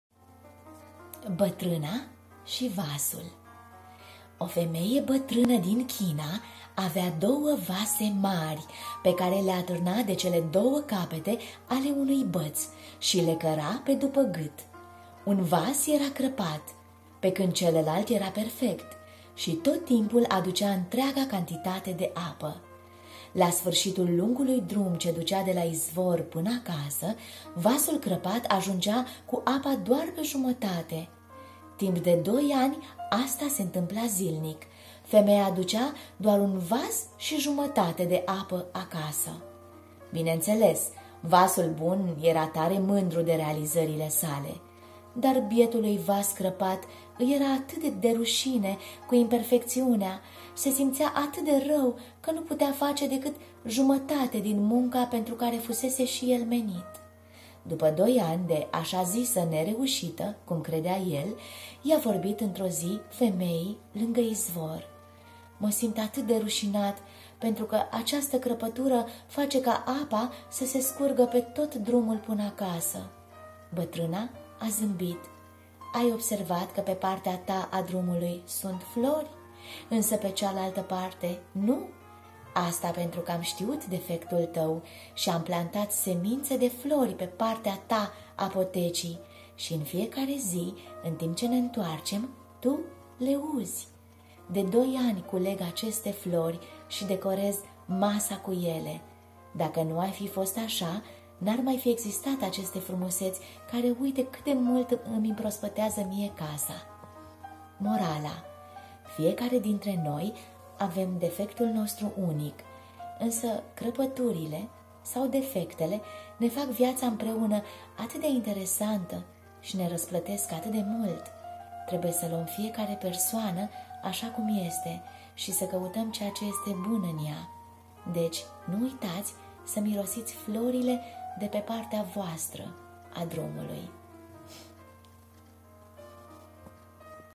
"Bătrâna și vasul" poveste
Bună dimineața în ziua Sfântului Gheorghe! La mulți ani sărbătoriților!
SfântulGheorghe să taie tot Raul din rădăcină și să doboare toți balauri din noi și din afara noastră!🌈
Primiți o Poveste ?
citesc povestiinizolare pentru o mai bună stare de spirit